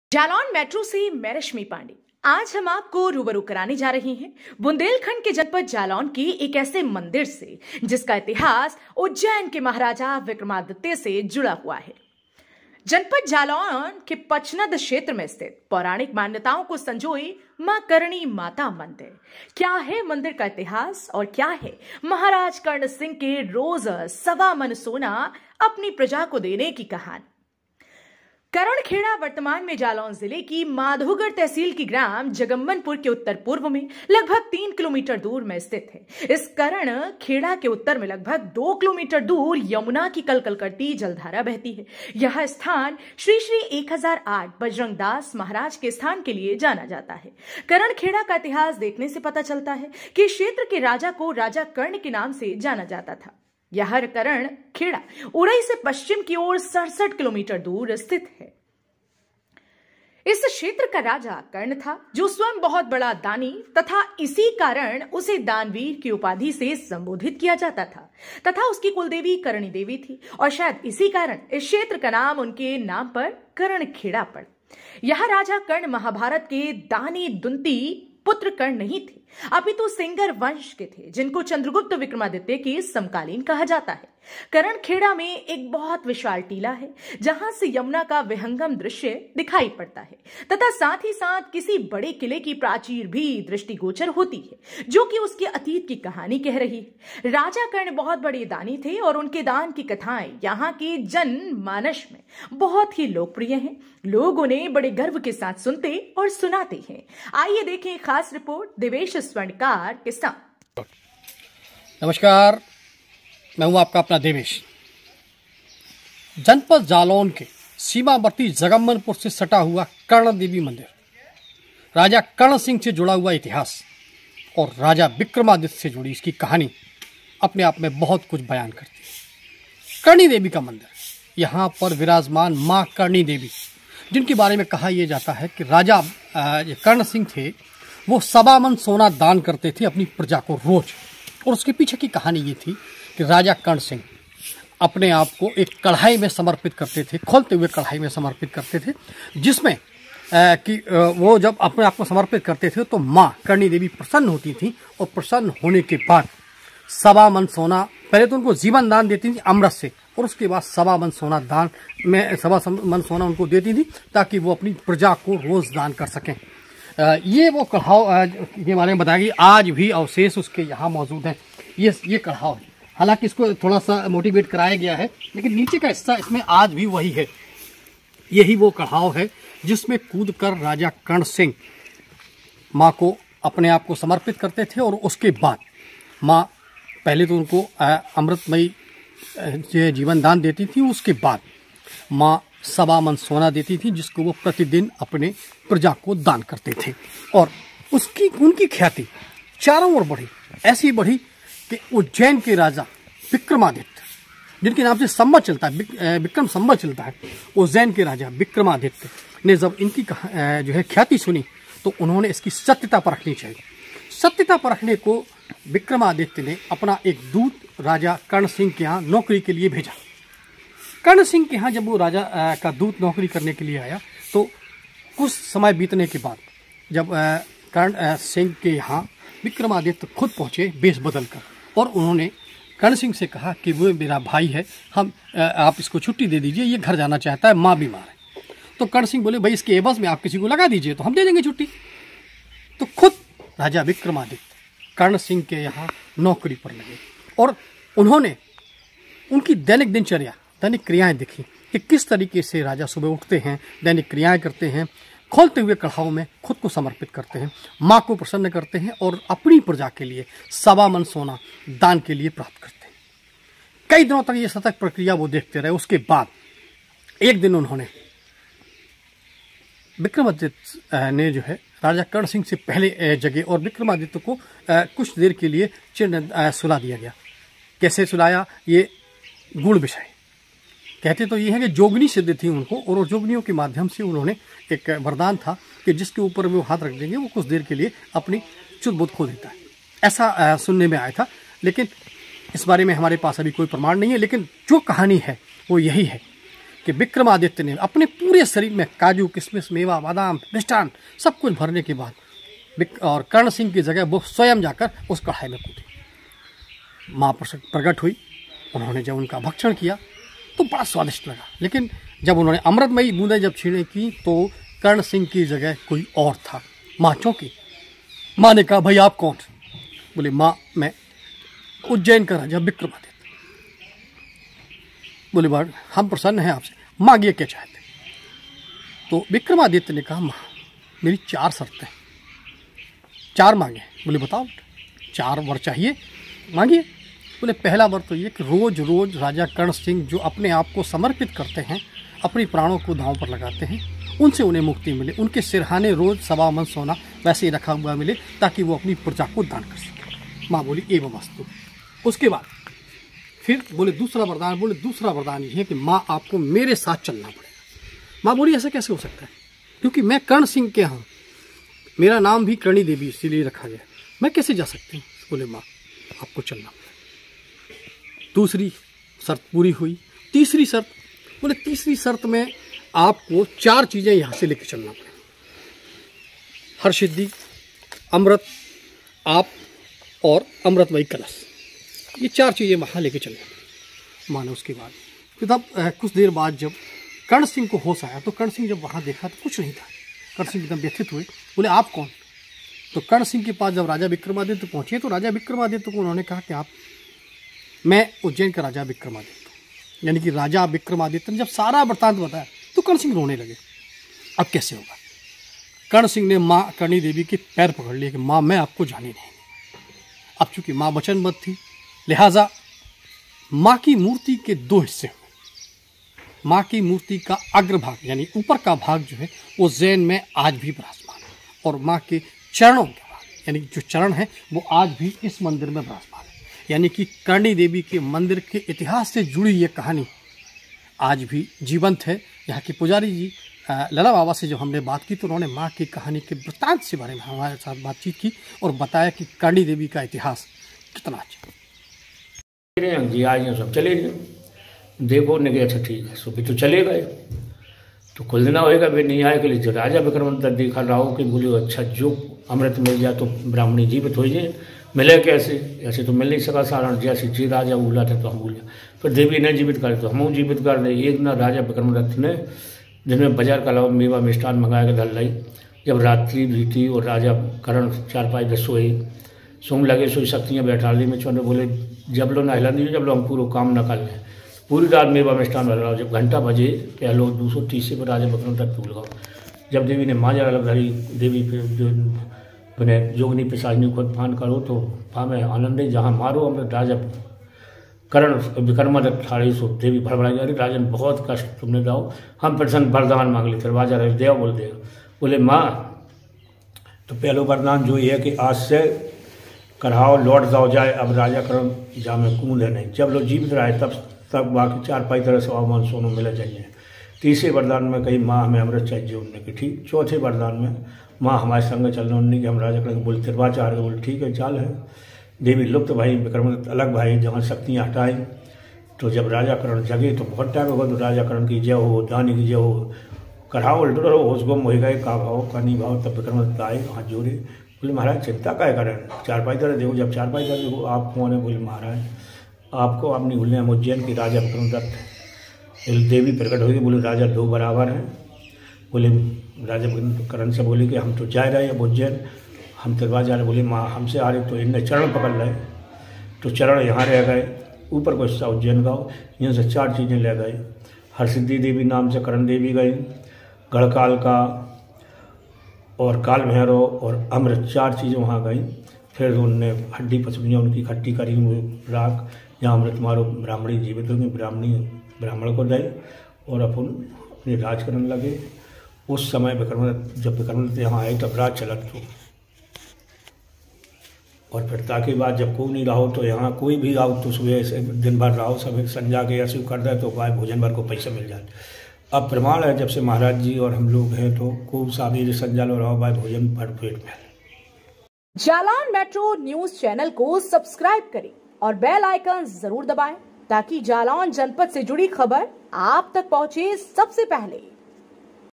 जालौन के पचनद क्षेत्र में स्थित पौराणिक मान्यताओं को संजोए मां करणी माता मंदिर का इतिहास....
जालौन

रिपोर्ट- देवेश कुमार स्वर्णकार

अद्भुत बुंदेलखंड

आज़ हम आपको रुबरु करानें जा रहें हैं बुंदेलखंड के जनपद जालौन के एक ऐसे मंदिर से जिसका इतिहास उज्जैन के महाराजा विक्रमादित्य से जुड़ा हुआ है
जनपद जालौन के पचनद क्षेत्र में स्थित पौराणिक मान्यताओं को संजोए मां करणी माता मंदिर
क्या है मंदिर का इतिहास और क्या है महाराज कर्ण सिंह के रोज सवा मन सोना अपनी प्रजा को देने की कहानी

करण खेड़ा वर्तमान में जालौन जिले की माधौगढ़ तहसील के ग्राम जगम्मनपुर के उत्तर पूर्व में लगभग 3 किलोमीटर दूर में स्थित है इस करण खेड़ा के उत्तर में लगभग 2 किलोमीटर दूर यमुना की कल कल करती जल धारा बहती है। यह स्थान श्री श्री 1008 बजरंगदास महाराज के स्थान के लिए जाना जाता है। करण खेडा का इतिहास देखने से पता चलता है कि इस क्षेत्र के राजा को राजा कर्ण के नाम से जाना जाता था। यह करण खेड़ा उरई से पश्चिम की ओर 67 किलोमीटर दूर स्थित है।

इस क्षेत्र का राजा कर्ण था जो स्वयं बहुत बड़ा दानी था तथा इसी कारण उसे दानवीर की उपाधि से सम्बोधित किया जाता था तथा उसकी कुल देवी कर्णी देवी थी और शायद इसी कारण इस क्षेत्र का नाम उसके नाम पर करण खेडा पड़ा। यह राजा कर्ण महाभारत के दानी दुन्ती पुत्र कर्ण नहीं थे अपितु सेंगर वंश के थे जिनको चन्द्रगुप्त विक्रमादित्य के समकालीन कहा जाता है। करण खेड़ा में एक बहुत विशाल टीला है जहाँ से यमुना का विहंगम दृश्य दिखाई पड़ता है तथा साथ ही साथ किसी बड़े किले की प्राचीर भी दृष्टिगोचर होती है जो कि उसके अतीत की कहानी कह रही है। राजा कर्ण बहुत बड़े दानी थे और उनके दान की कथायें यहाँ के जन मानष में बहुत लोक प्रिय है लोग उन्हें बड़े गर्व के साथ सुनते और सुनाते हैं।

आइए देखें खास रिपोर्ट देवेश कुमार स्वर्णकार के साथ